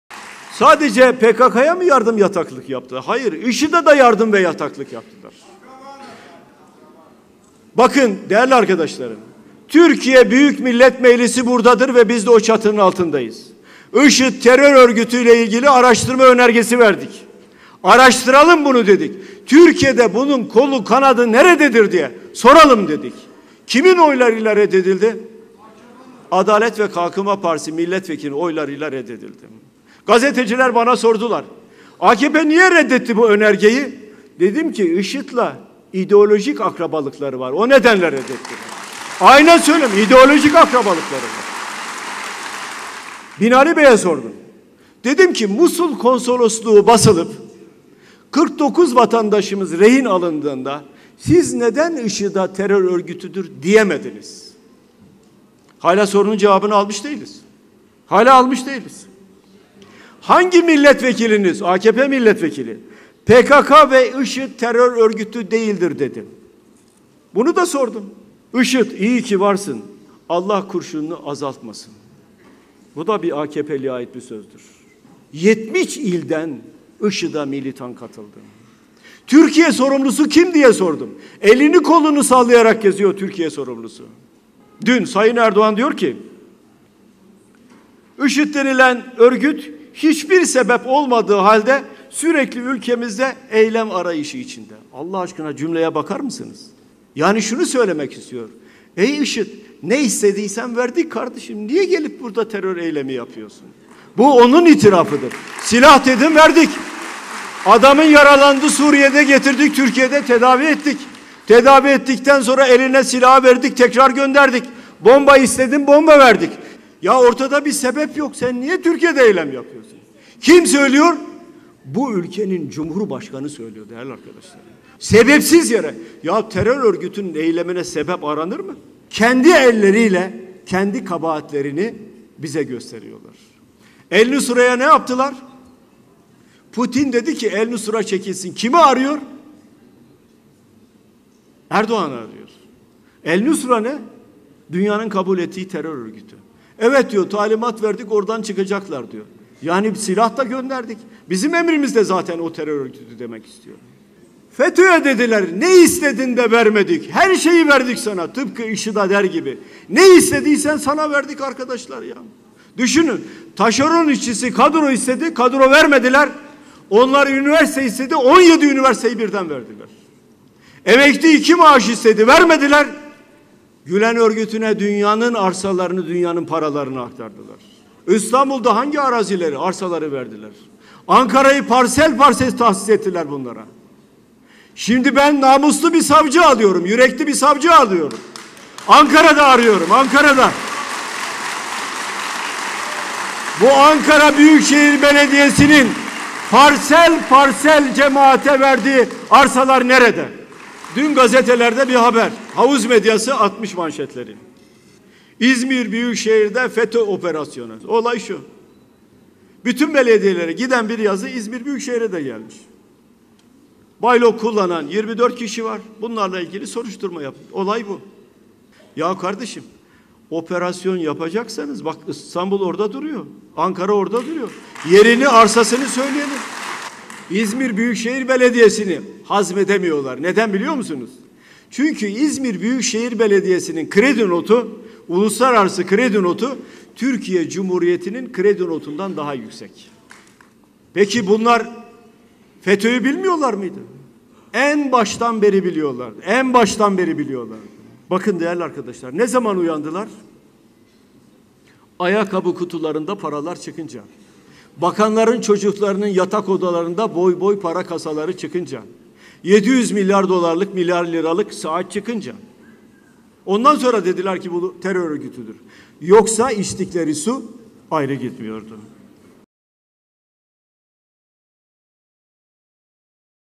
Yürekli Bir Savcı Arıyorum!
Bunlar sadece PKK'ya yardım ve yataklık yapmadılar, IŞİD'e de yardım ve yataklık yaptılar. IŞİD terör örgütü hakkında araştırma önergesi verdik, IŞİD'le ideolojik akrabalıkları olduğu için, önerge AKP'nin oylarıyla reddedildi!